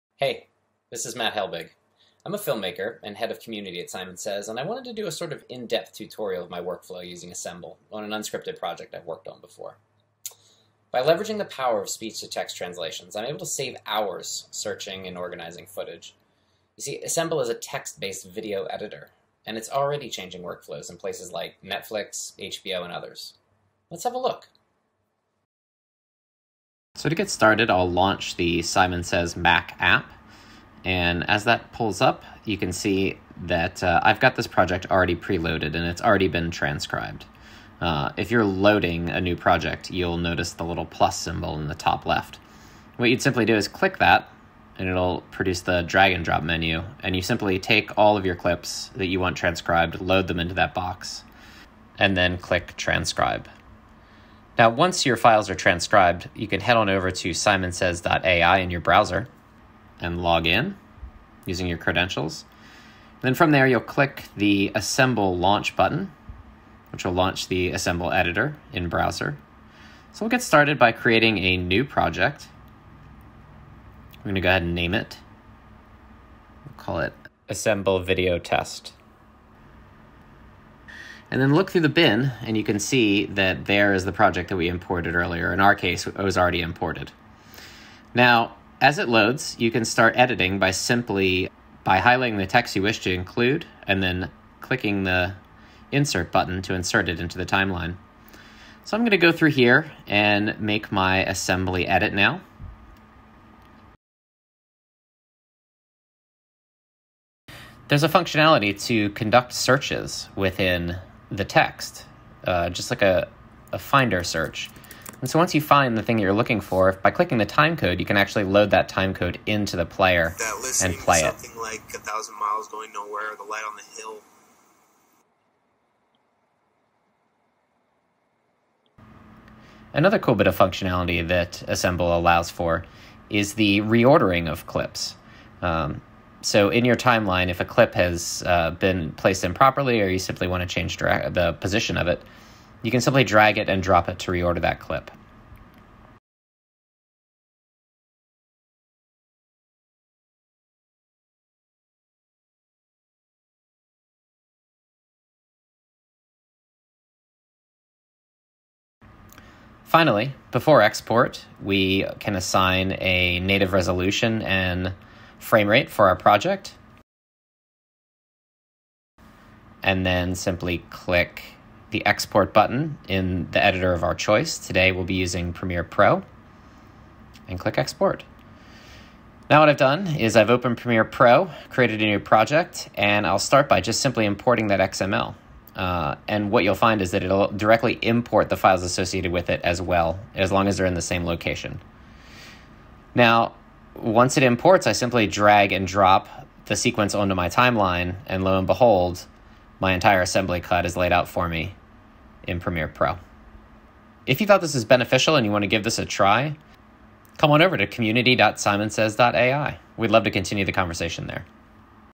Workflow Walkthrough: Simon Says/Assemble/Premiere Pro
This is an example of post-production workflow on unscripted projects using the Simon Says Mac App, Assemble and Premiere Pro.

Assemble is the first of its kind text-based video editor. Using AI, Assemble enables editors and producers to speed through the assembly stages of their projects.

Check out community.simonsays.ai to learn more.